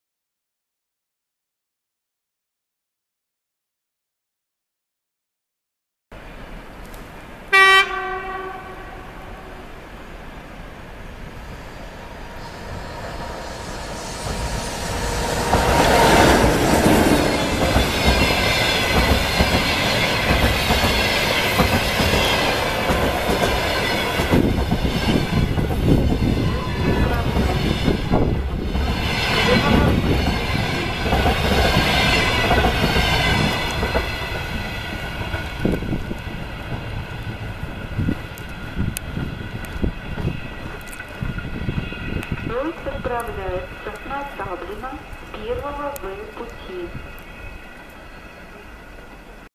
ЭП2К с поездом 081А Санкт-Петербург – Белгород проходит станцию Колпино
Электровоз  ЭП2К с поездом 081А Санкт-Петербург – Белгород проходит станцию Колпино.
Октябрьская железная дорога, Санкт-Петербург.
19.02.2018

Electric locomotive EP2K with train "Saint Petersburg — Belgorod".
St.Petersburg region, Russia.